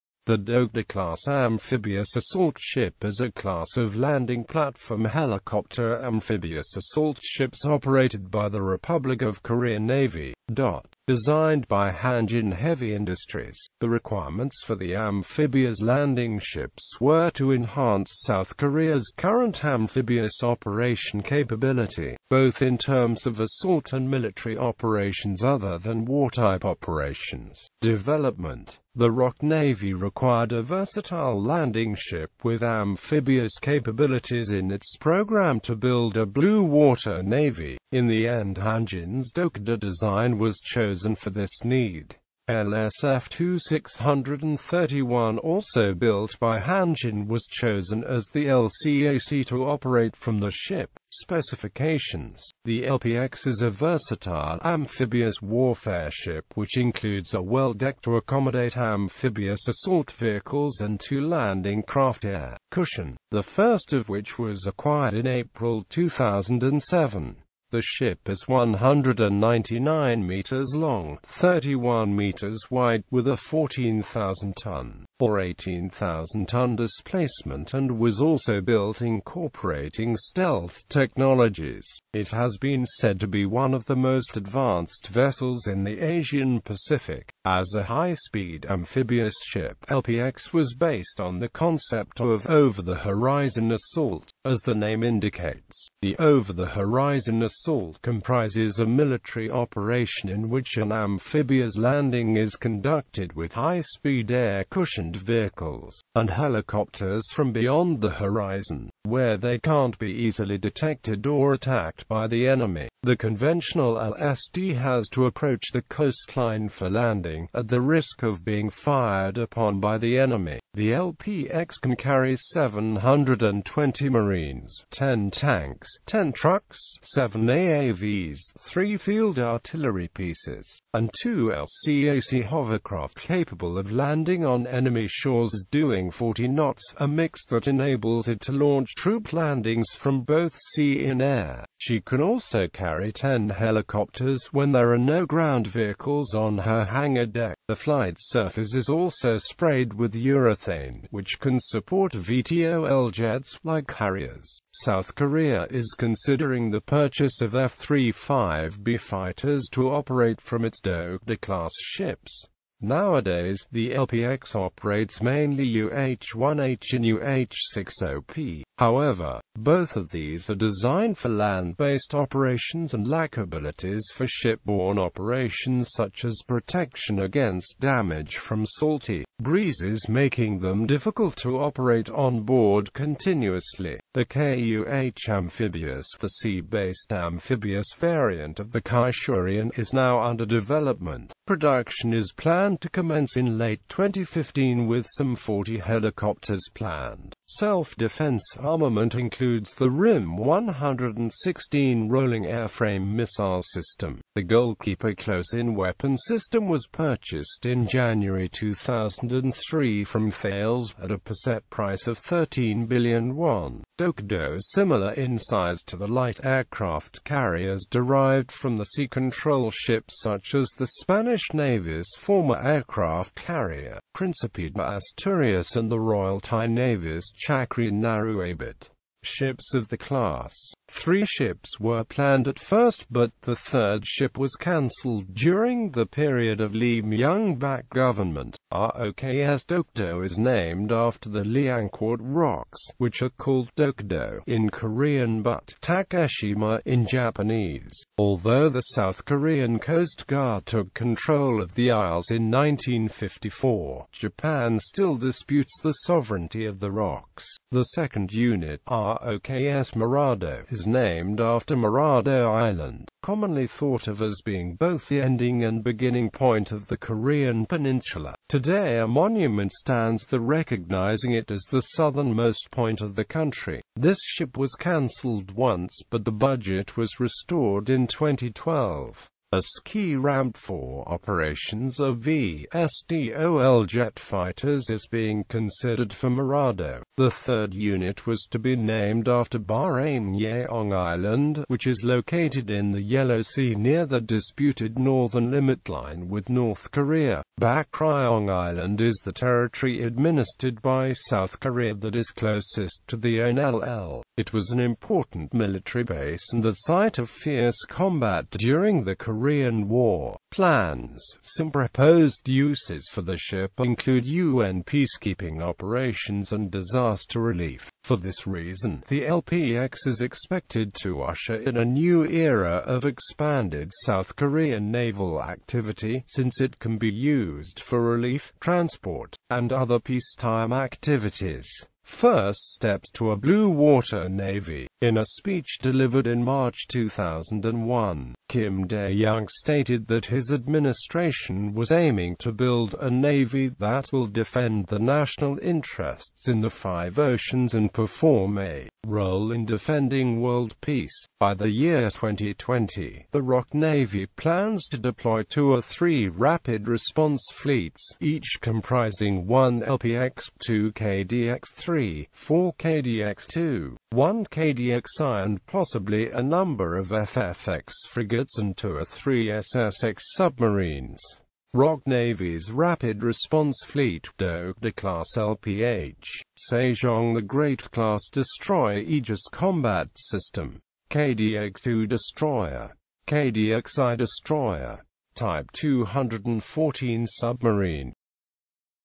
Dokdo-class amphibious assault ship
Ad-free videos.

The Dokdo-class amphibious assault ship is a class of Landing Platform Helicopter amphibious assault ships operated by the Republic of Korea Navy .Designed by Hanjin Heavy Industries, the requirements for the amphibious landing ships were to enhance South Korea's current amphibious operation capability, both in terms of assault and military operations other than war type operations.

---Image-Copyright-and-Permission---
About the author(s): Mass Communication Specialist 2nd Class Greg Mitchell
---Image-Copyright-and-Permission---